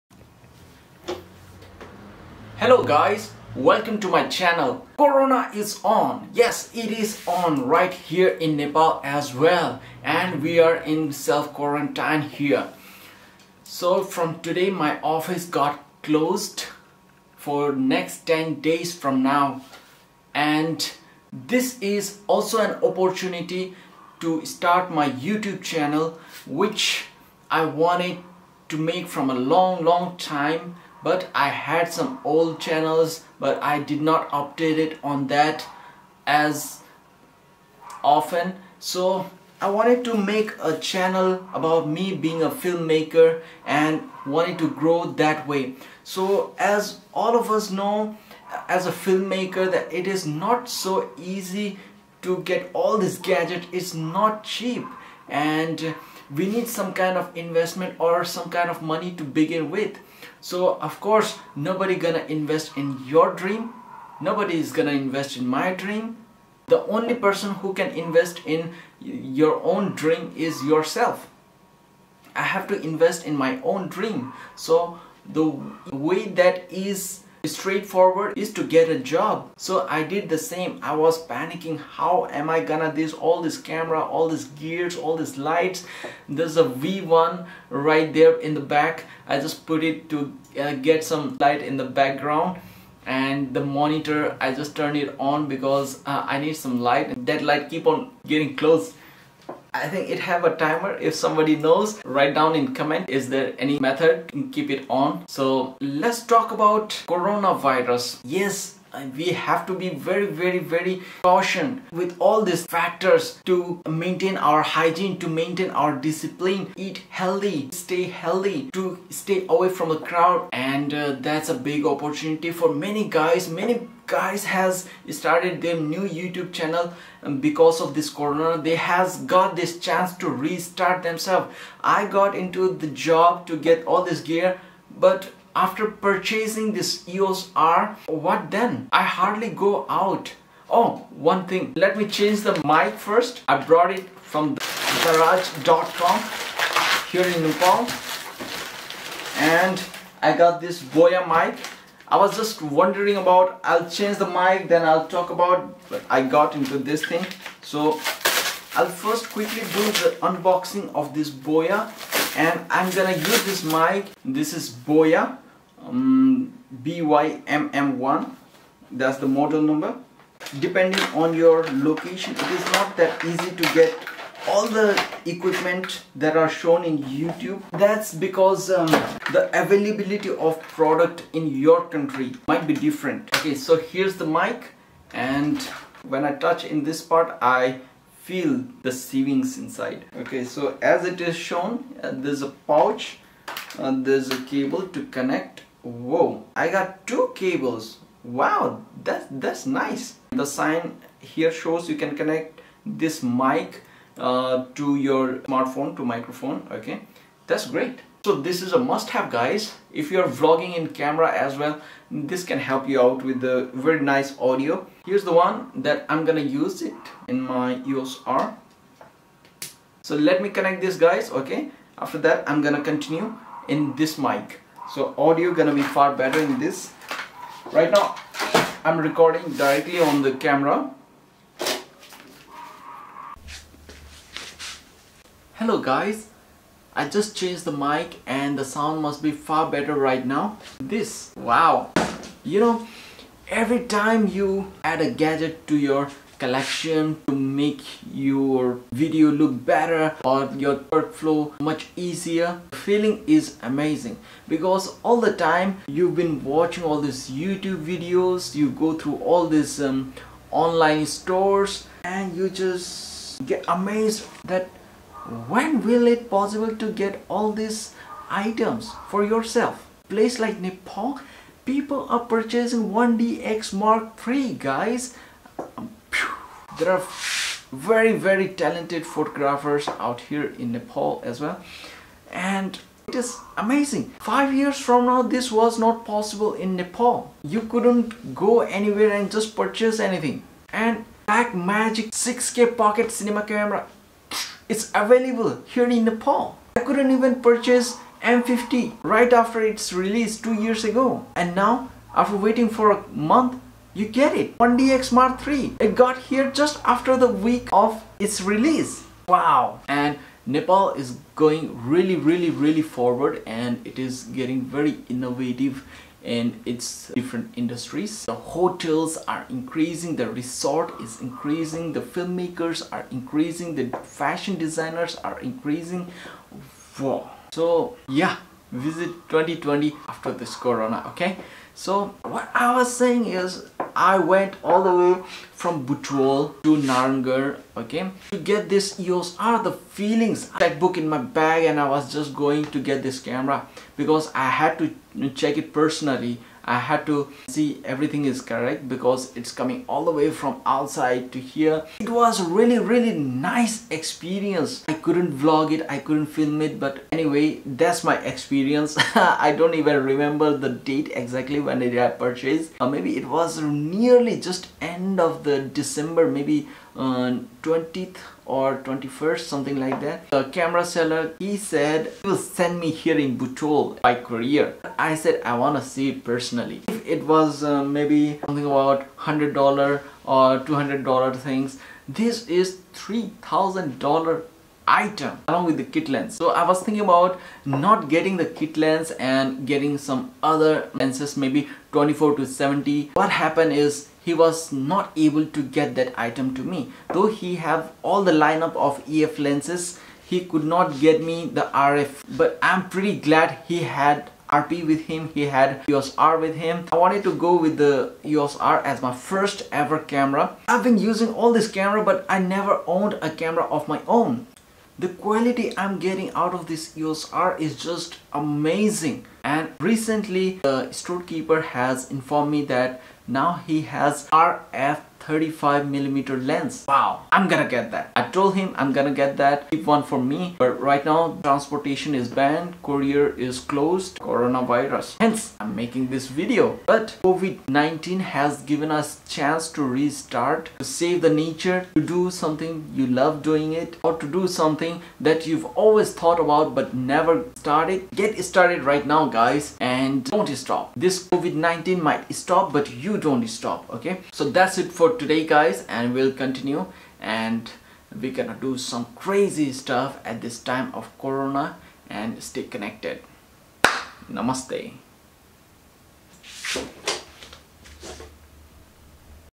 Getting Started in YouTube at COVID-19 Lockdown with EOS R in Nepal
Hello guys this is my semi introduction on sharing my experience on how I got my first camera ever journey.

Starting my New YouTube channel with zero audience. Now its time I be more creative.